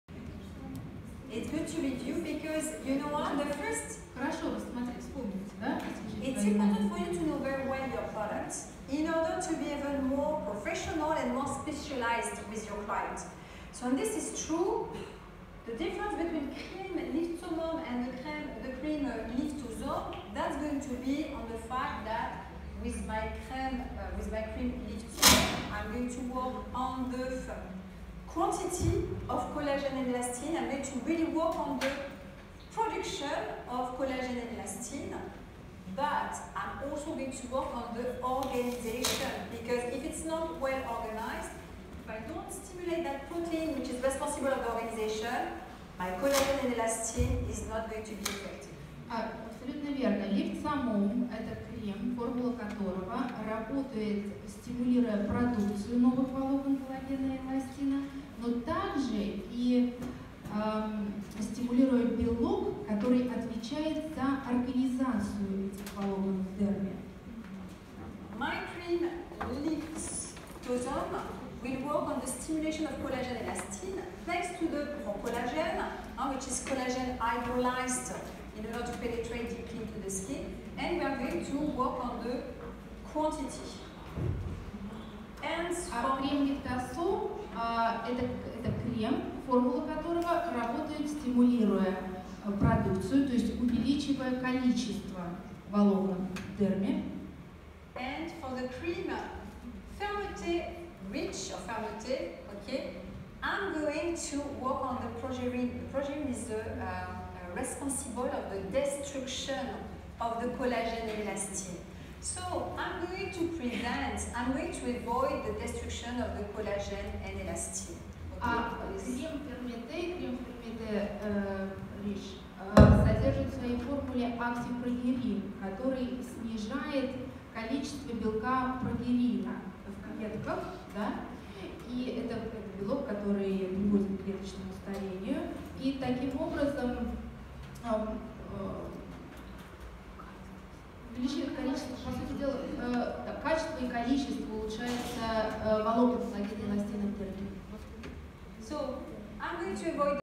Разница в кремах Гино (Guinot) для укрепления кожи лица
Линия Упругость включает в себя 3 ключевых позиции : крем Fermete Lift для снижения скорости разрушения коллагена за счет актипрогерина, плюс наполнения кожи 2 видами гиалуроновой кислоты ,
Litfosome - для синтеза нового коллагена и внесение в кожу его гидрализованной формы. И Lift Summum - для синтеза и структуризации коллагена, формирование структуры дермы.
3d омоложение от сети салонов Мишель Экзертье